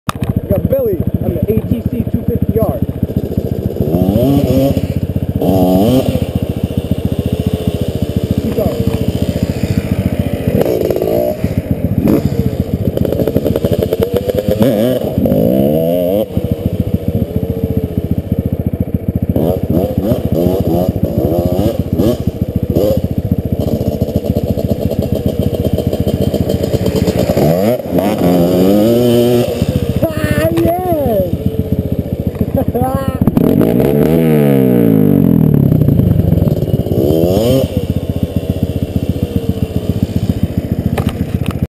ATC250R Drifting
william16339 drifting !!! almost fell back at end